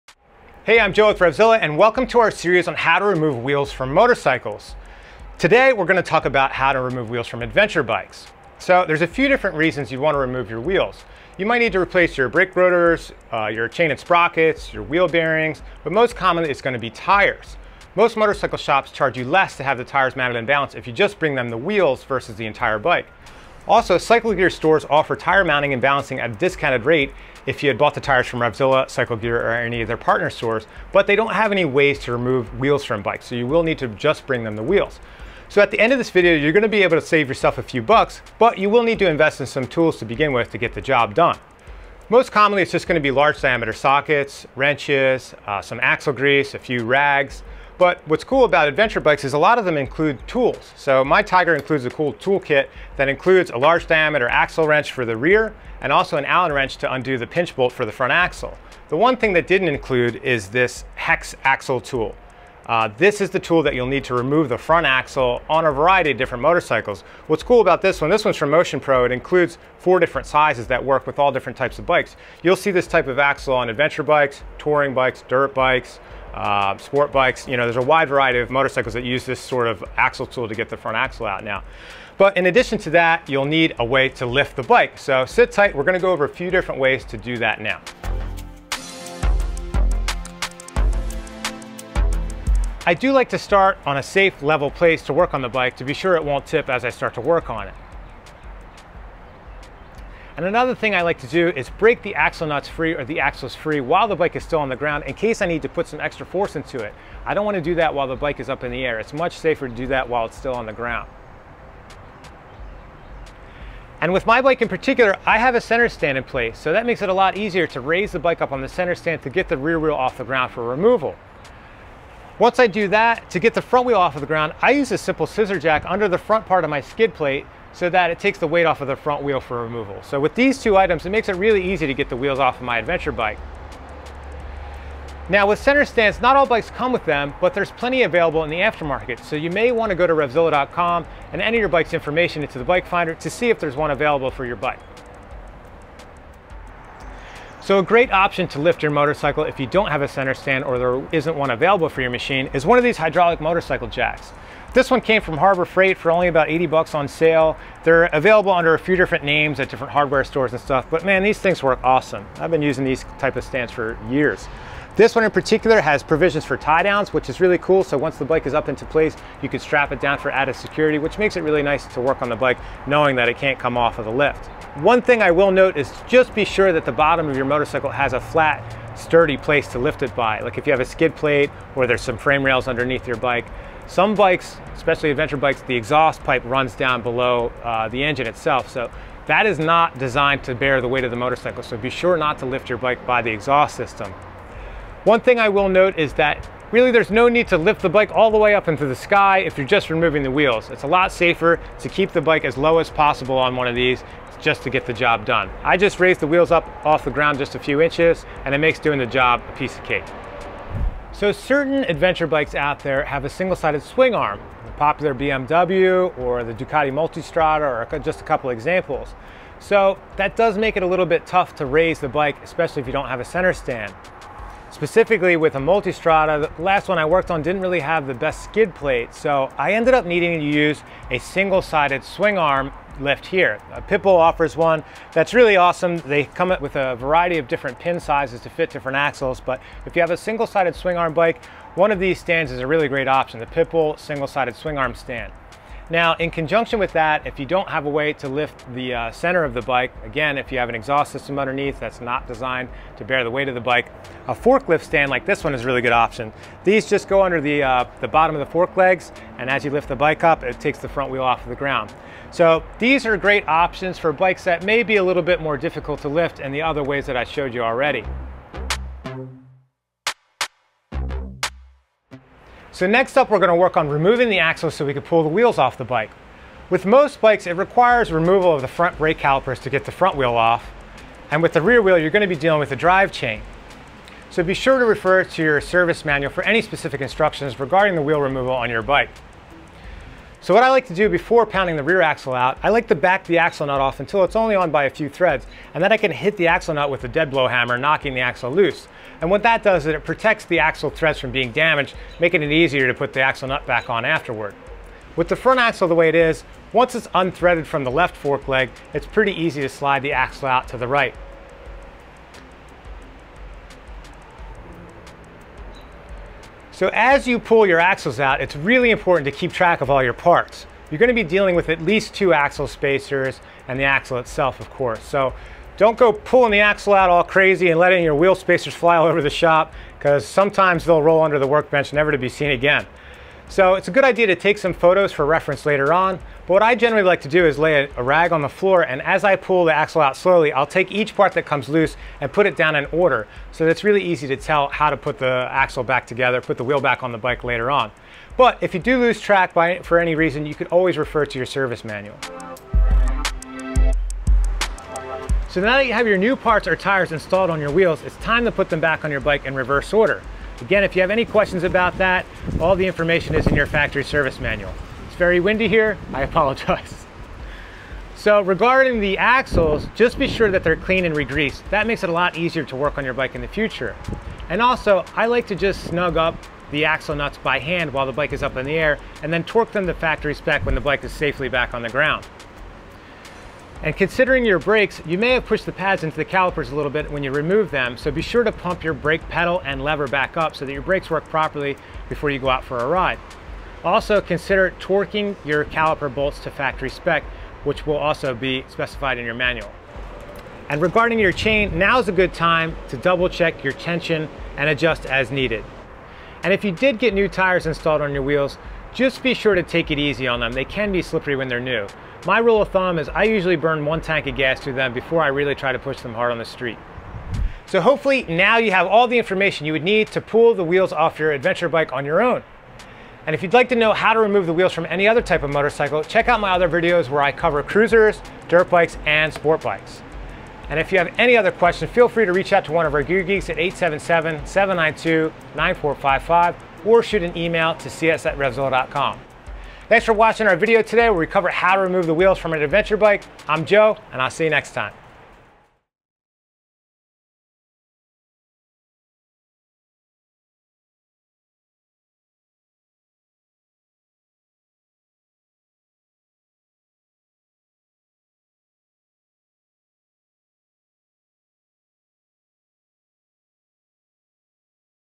How To Remove Motorcycle Wheels | ADV Bikes
Lifting your ADV bike and removing its wheels doesn't need to be a chore. Joe Zito's here with the essential information you need to get the job done. Whether you're swapping tires, adjusting suspension, or simply getting the bike off the ground, follow along with Zito for a safe and stress-free experience.